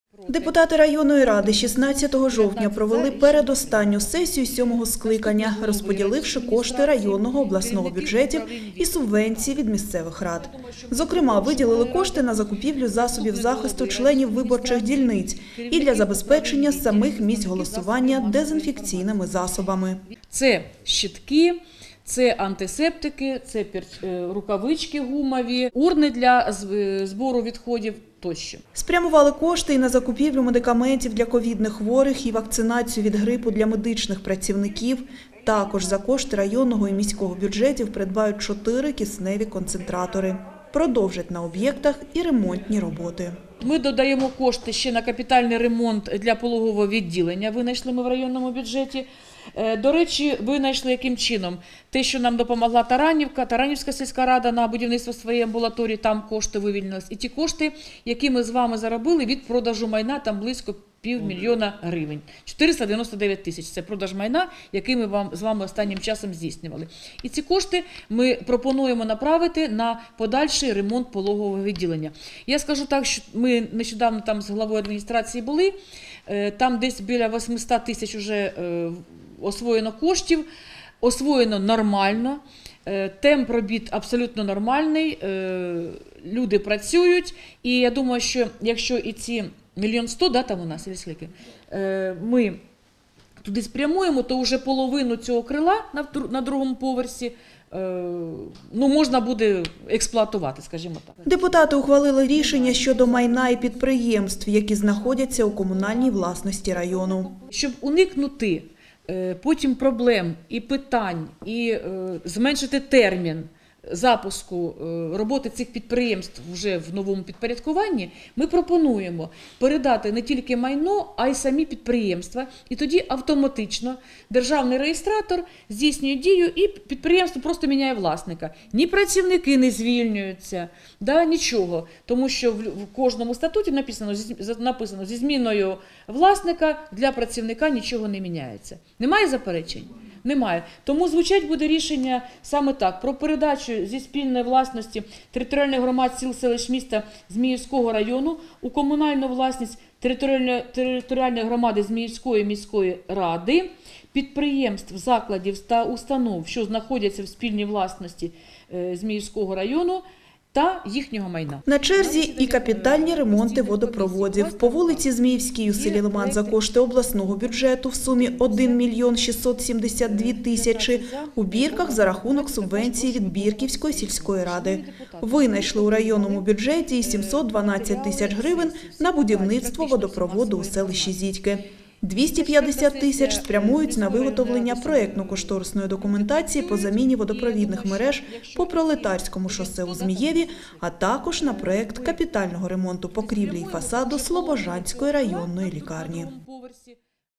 Капітальні ремонти і медикаменти. На що спрямували районні кошти
На черговій сесії районної ради депутати виділили кошти на закупівлю засобів захисту членів виборчих дільниць, і для забезпечення самих місць голосування дезінфекційними засобами. Частину коштів спрямували на закупівлю медикаментів для ковідних хворих і вакцинацію від ГРИПу для медичних працівників, а також  - кисневих концентраторів (окрім районного бюджету, на їх придбання кошти виділила і Зміївська міська рада). Продовжать і ремонти майбутнього пологово-гінекологічного відділення у Зміївській ЦРЛ, на ці цілі винайшли ще більше мільйона гривень. Також за субвенції місцевих рад, районного і обласного бюджетів ремонтуватимуть і будуватимуть водопроводи у селах Бірки, Лимані і селищі Зідьки. Виділили кошти і на розробку проєктів капітального ремонту покрівлі і фасаду КНП "Слобожанська районна лікарня" та водопровідних комунікацій на мікрорайоні м.Змієва по Пролетарському шосе.